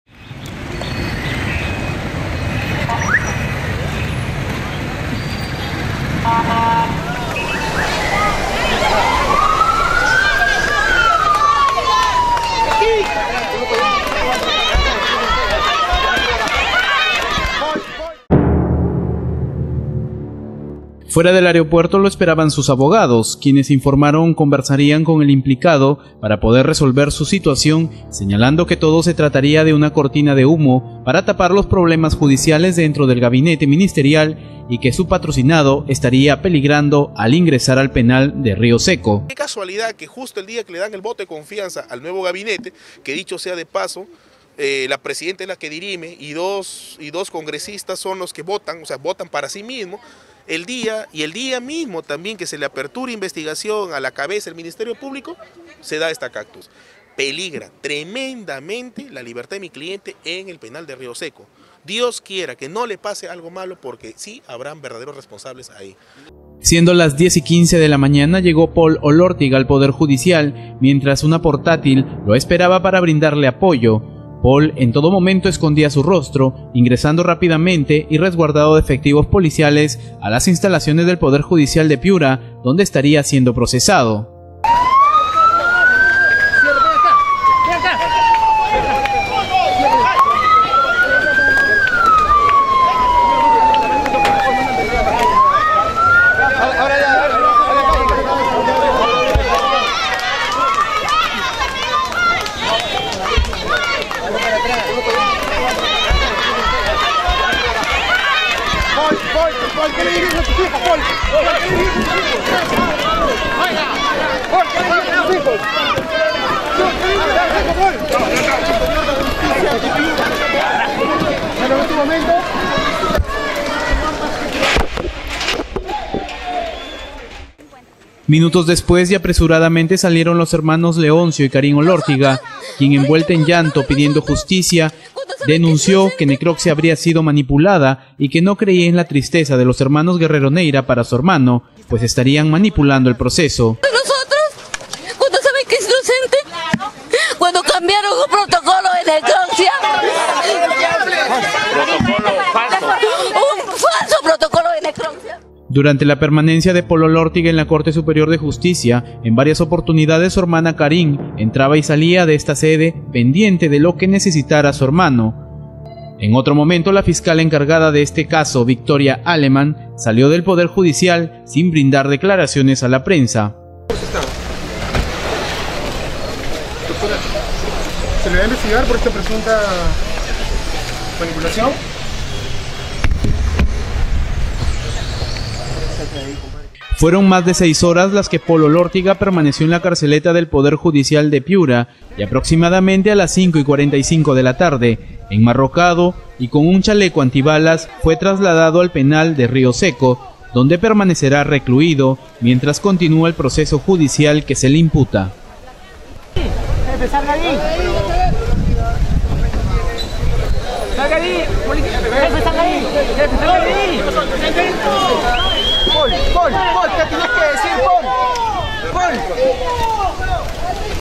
Paùl Olortiga Contreras Fue llevado al Penal de Rio Seco - UCV Noticias Piura
UCV Noticias Ediciòn Regional - UCV Satelital Filial Piura
Paul Olòrtiga Contreras , Presunto asesino de la fallecida Cantante de Corazòn Serrano , Llego esta mañana a la Base Aèrea N 07 de Piura, resguardado de un fuerte contigente Policial